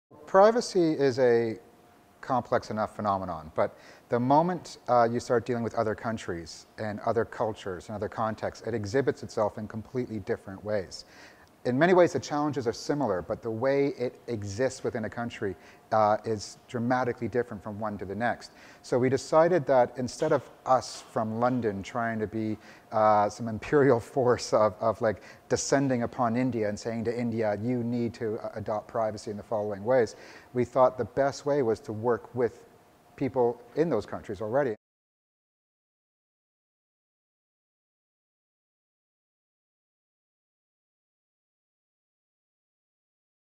PI Network - Formation
The Privacy International Network has been working since 2008 to build a global privacy movement. Hear from PI Executive Director Gus Hosein on why we took this step.